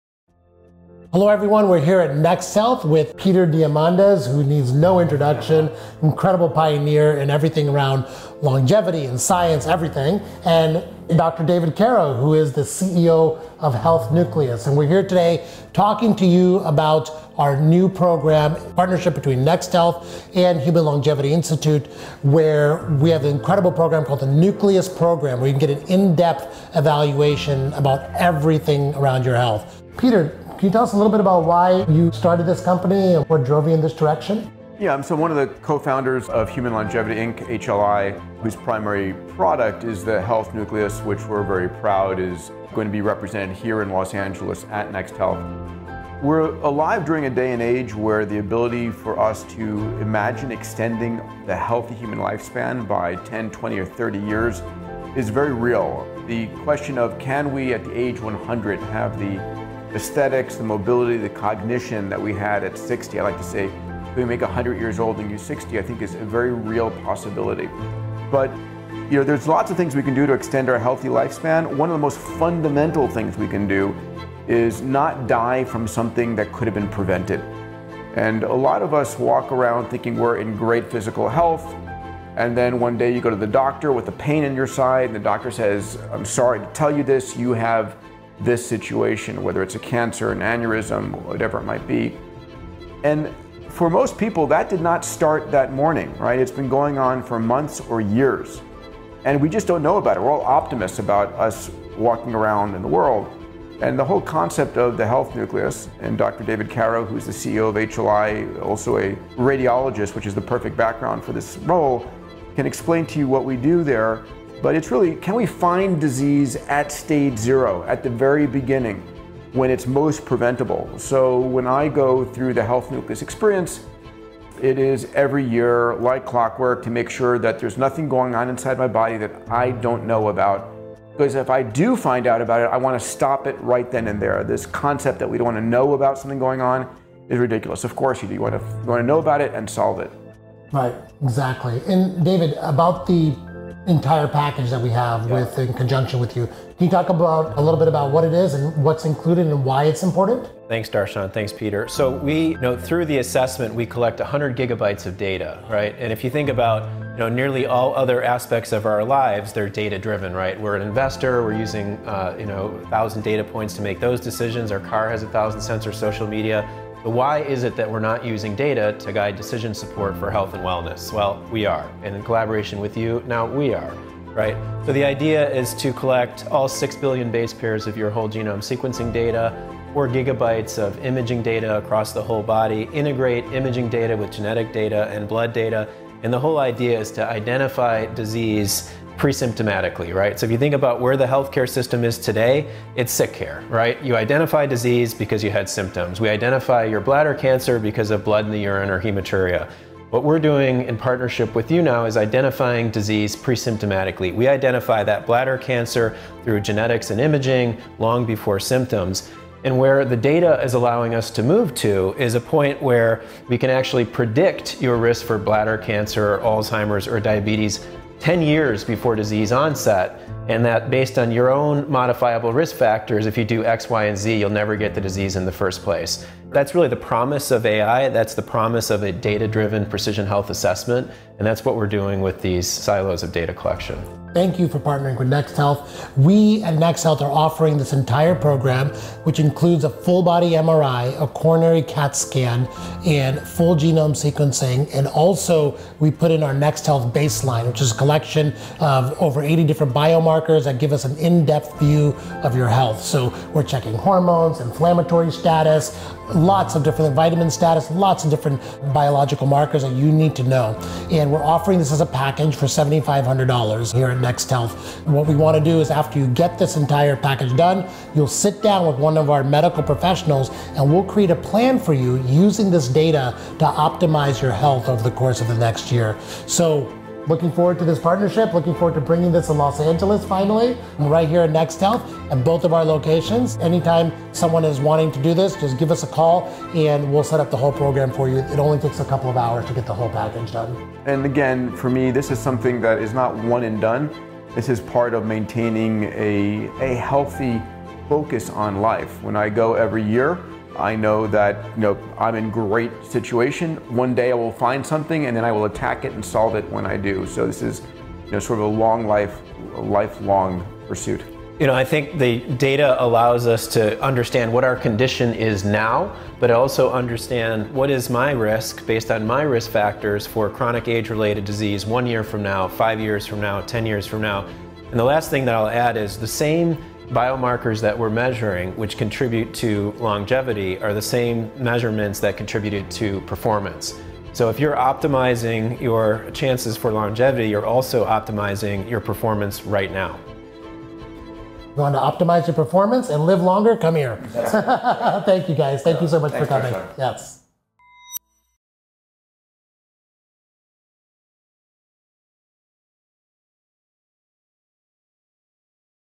Introducing FULL Genome Sequencing
We are excited to introduce our new cutting-edge and life-changing program, FULL GENOME SEQUENCING. This partnership with Human Longevity Insitute ushers in a new day and age for what we call healthcare.

Listen in as Dr. Darshan Shah, CEO of N|H, Peter Diamandis, and David S. Karrow, CEO of Human Longevity, Inc., talk about this amazing advancement in medical technology and how it's changing lives.

Locations: Westfield At Century City | West Hollywood | New York City